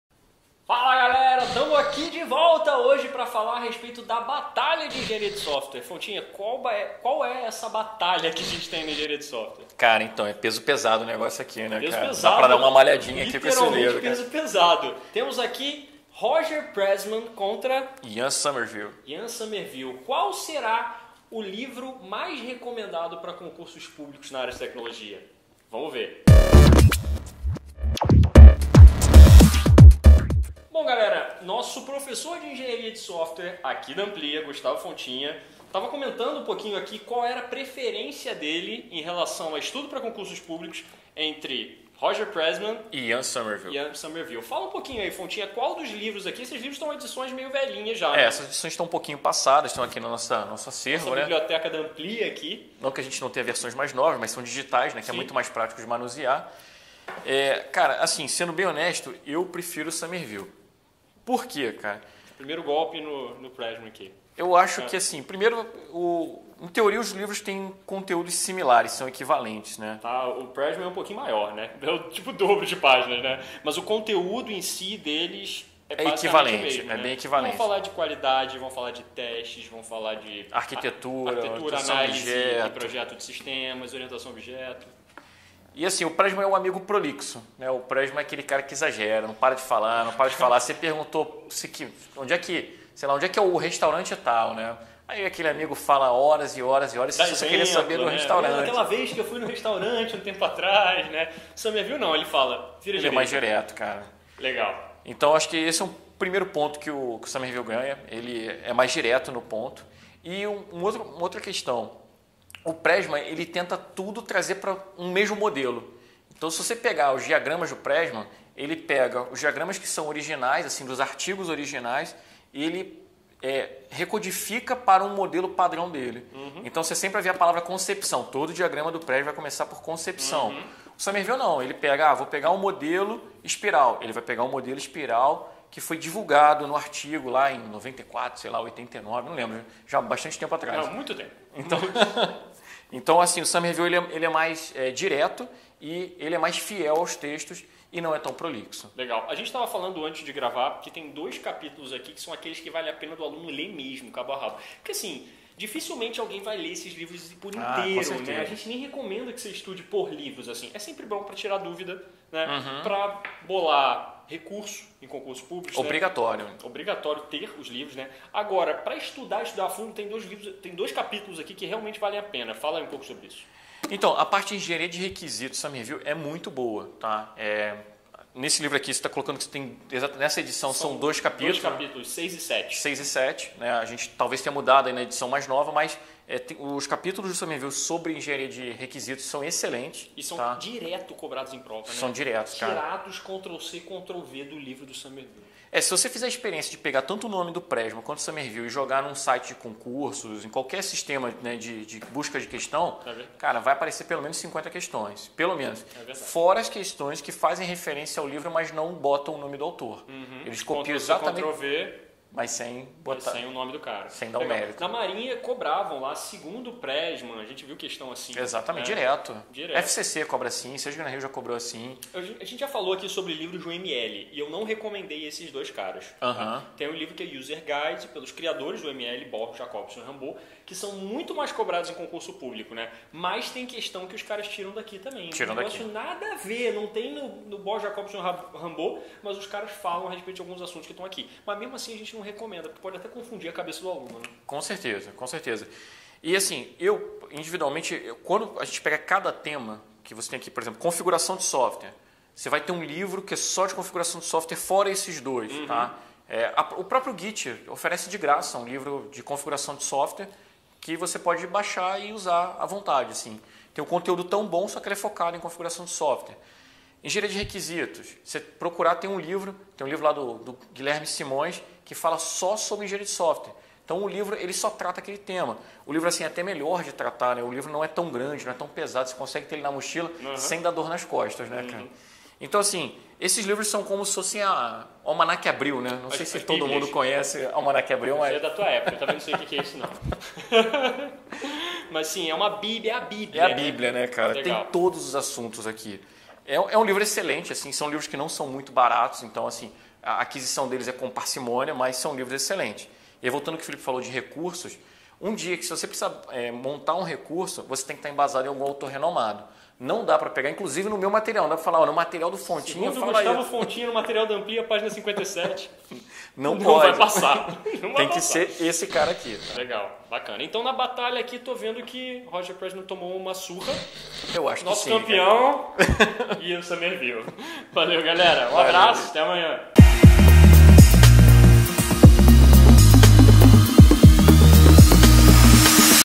Engenharia de Software para Concursos: Roger Pressman x Ian Sommerville | 44 de 100
No vídeo de hoje tivemos uma batalha de peso: Roger Pressman x Ian Sommerville - Qual dos dois é o melhor quando se trata de livros para estudar Engenharia de Software para Concursos? Confira a análise completa no vídeo!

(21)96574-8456